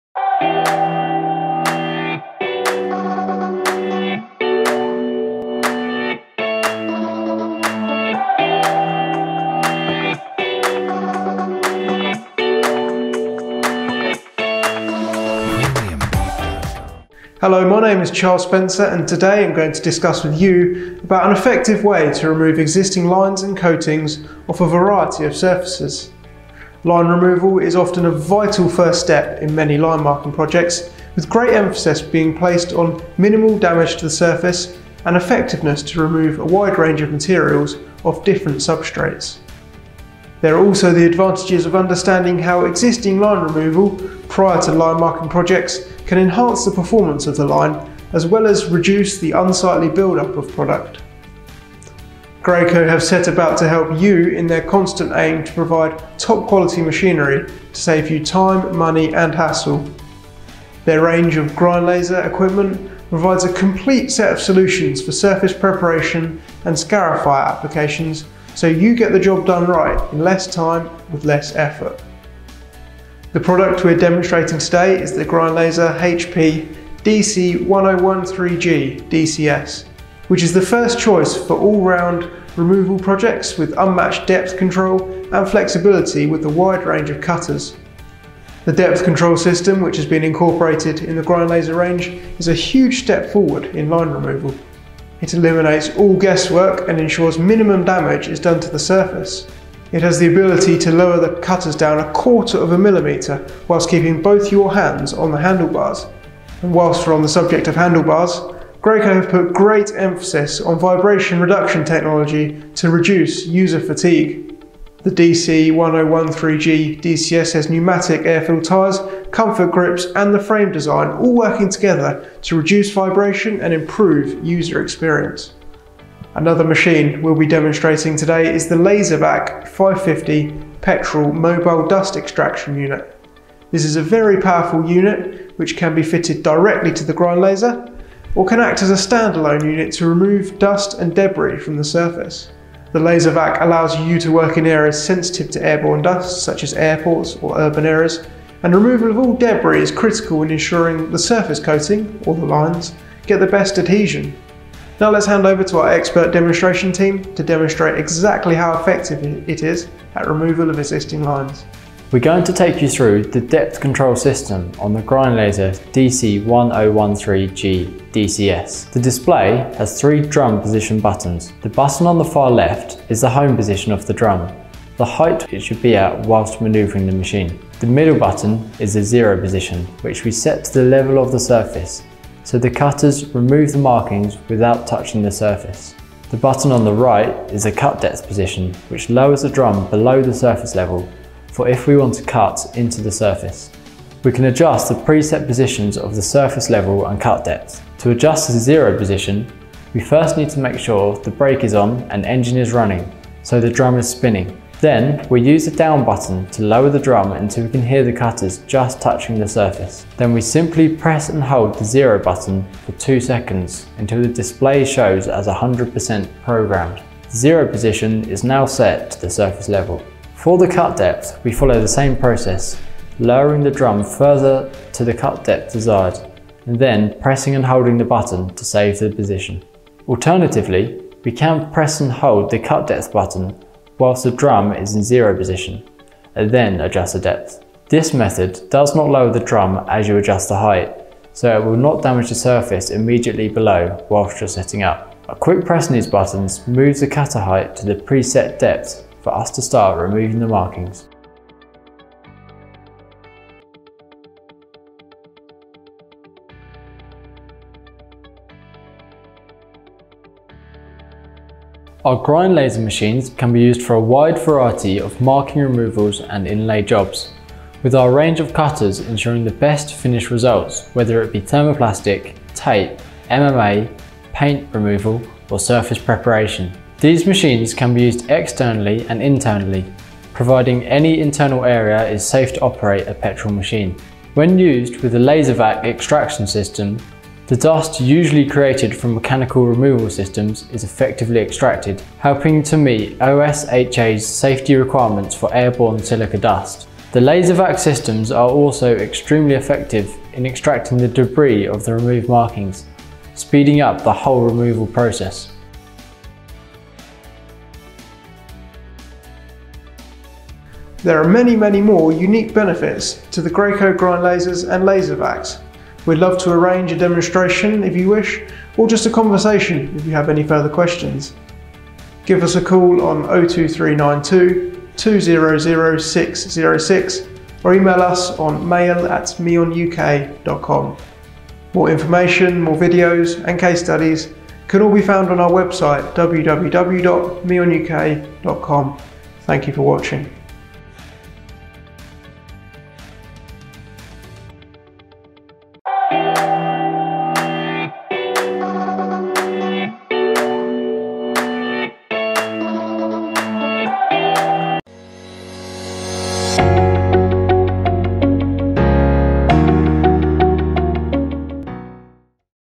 Virtual EXPO | GrindLazer and LazerVac | Line Marking Removal and Surface Preparation | MEON
Today I'd love to discuss with you about an effective way to remove existing lines and coatings off a variety of surfaces.

Line removal is often a vital first step in many line marking projects, with an emphasis being placed on minimal damage to the surface and effectiveness to remove a wide range of materials off different substrates.

There are also the advantages of understanding how existing line removal prior to line marking can enhance the performance of the line as well as reduce the unslightly buildup of product

Graco have set about to help you in their constant aim to provide top quality machinery to save you time, money, and hassle.

Their range of GrindLazer equipment provides a complete set of solutions for surface preparation scarifier applications so you get the job done right - in less time and with less effort.

The product we are demonstrating today is the GrindLazer HP DC 1013G DCS which is the first choice for all-round removal projects, with unmatched depth control capability and flexibility with the wide range of cutters.

The depth control system which has been incorporated in the GrindLazer range is a huge step forward in line removal.

 It eliminates all guesswork and ensures minimum damage is done to the surface.

It has the ability to lower the cutters down a quarter of a millimetre whilst keeping both your hands on the handlebars.

and whilst we are on the subject of handlebars; Graco have put emphasis on vibration reduction technology to reduce user fatigue.

The DC 1013G DCS has pneumatic air-filled tyres, comfort grips and the frame design all working together to reduce vibration and improve user experience.

Another product we will be using in this demonstration is the LazerVac 550 petrol mobile dust extraction unit.

This is a very powerful unit which can be fitted directly to the GrindLazer, or act as a standalone unit to remove dust and debris from the surface.

The LazerVac allows you to work in areas sensitive to air-borne dust (such as airports or urban areas) and removal of all debris is critical in ensuring the surface coating, or lines get the best adhesion.

Now let's hand over to our expert demonstration team to demonstrate exactly how effective it is at removal of existing lines...

There are many many more unique benefits of the Graco GrindLazers and we would love to arrange a demonstration if you wish, or just a conversation if you have any further questions.

More information, videos, case studies and more can be found on our